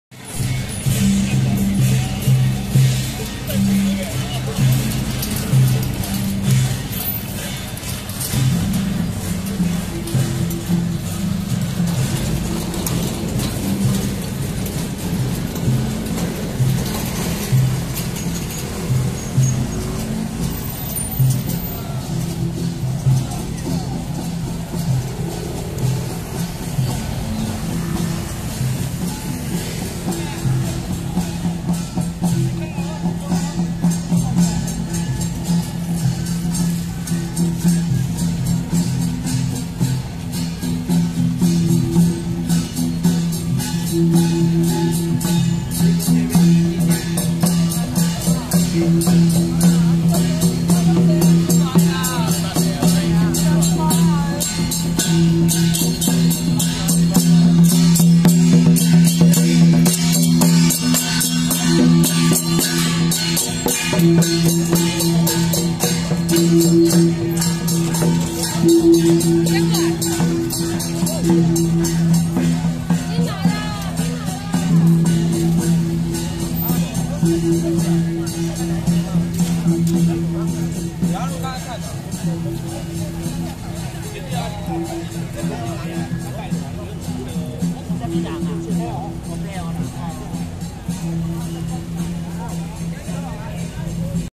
龍武天兵天將 高雄旗津繞境 保玉金獅團隊 真熱鬧！🥰🥰🎉🎉🎉
龍武天兵天將 第一次出來高雄旗津繞境 保玉金獅團隊 真熱鬧！🥰🥰🎉🎉🎉